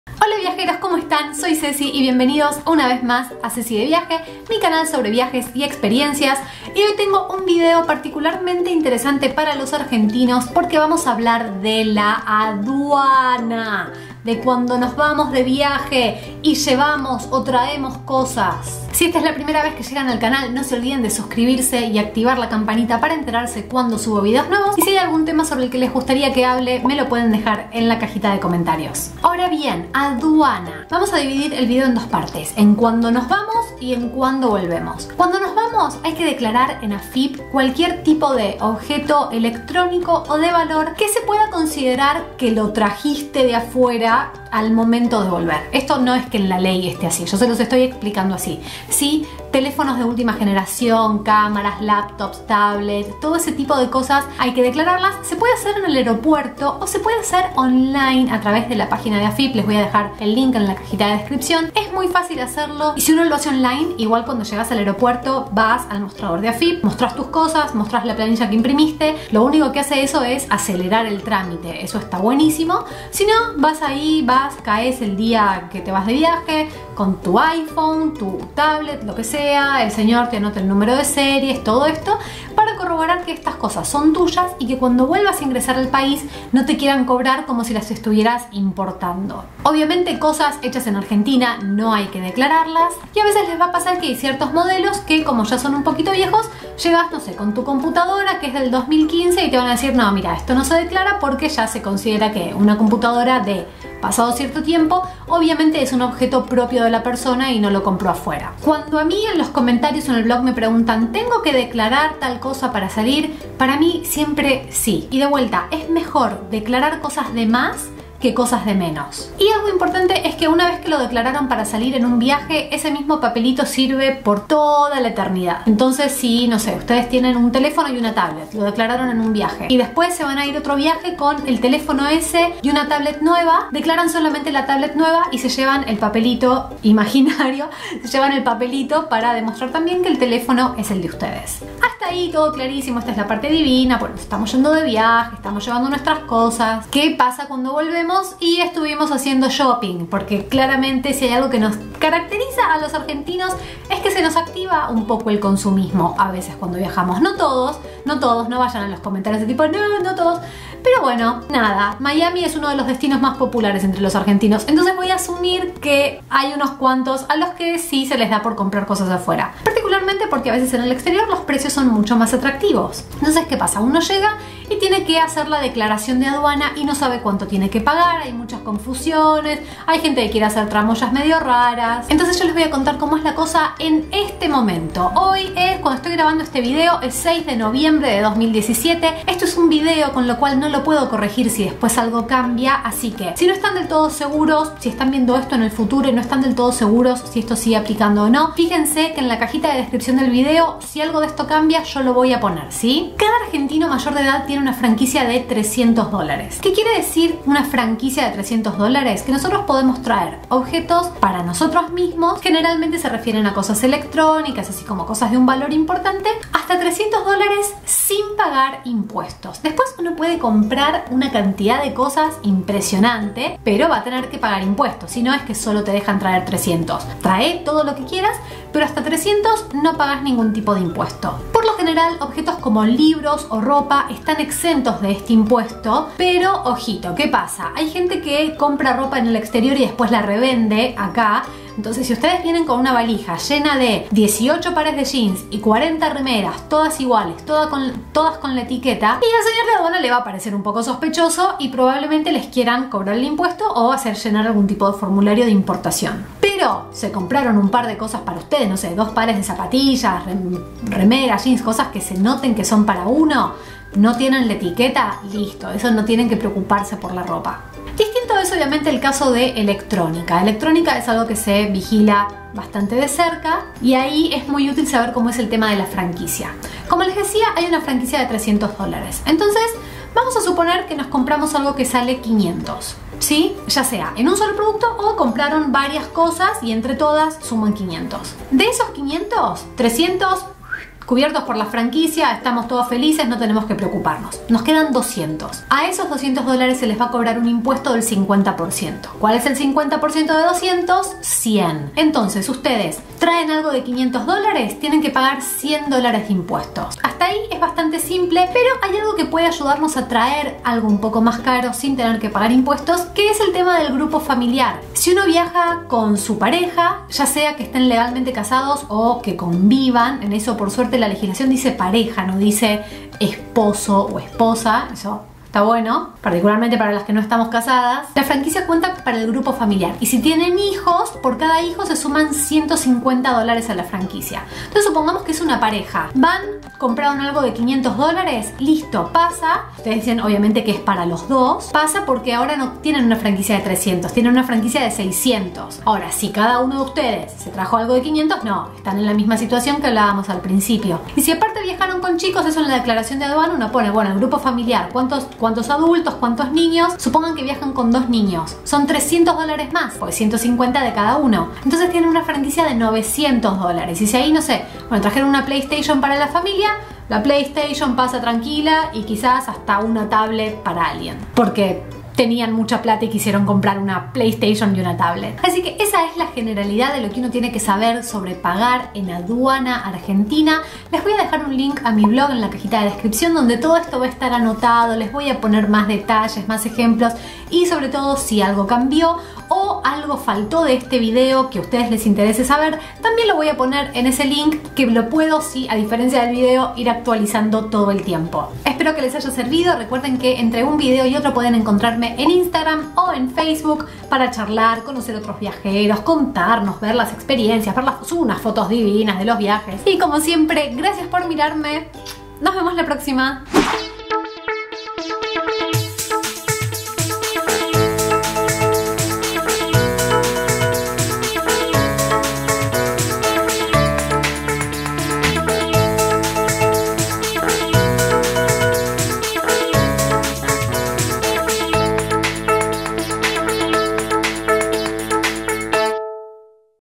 TRAER CELULARES DEL EXTERIOR A ARGENTINA + ADUANAS | Ceci de Viaje
🇦🇷 ¿Cómo traer un celular de Estados Unidos, Chile o cualquier otro país del exterior a Argentina? ¿Qué hay que pagar?
¿Qué se declara en la aduana argentina? ¿Qué no? ¿Cuánto se paga de impuestos? Vamos a ponerlo en claro, que viajeros prevenidos valen por dos!
🚨 IMPORTANTE! 🚨

CON QUÉ GRABO:
- Canon G7X
- Canon EOS M6
- GoPro Hero 5
- O la cámara de mi teléfono :)